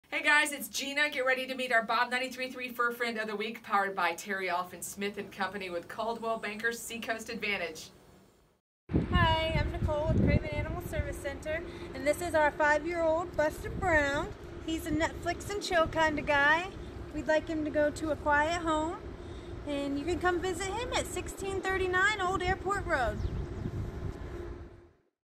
BUSTER BROWN- Bob 93.3 Fur Friend of the Week
Buster Brown is our Fur Friend of the Week powered by Terri Alphin Smith & Co. with Coldwell Banker Sea Coast Advantage. He is with Craven Pamlico Animal Services.